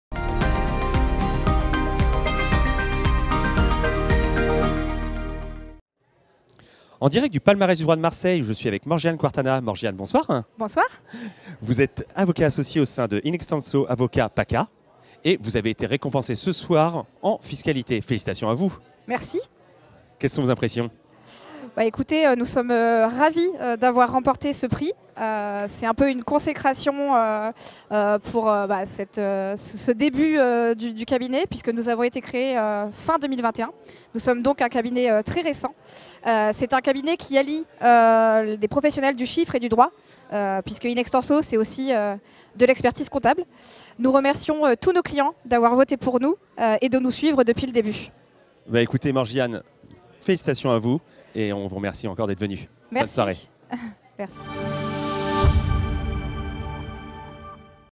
Palmarès du Droit Marseille 2024 - In Extenso Avocats PACA
Interview de Morgiane Quartana, Avocat associée en droit fiscal - In Extenso Avocats PACA